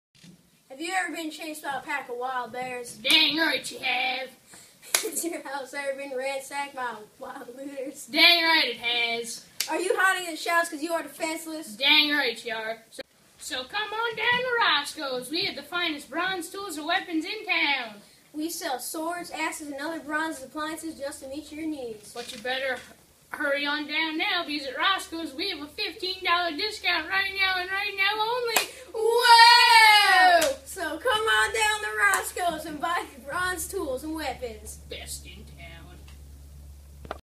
Mesopotamia Commercials by Patrick and Michael O
Students in Mr. D's 6th grade world history class made short TV commercials for important Mesopotamian inventions and achievements.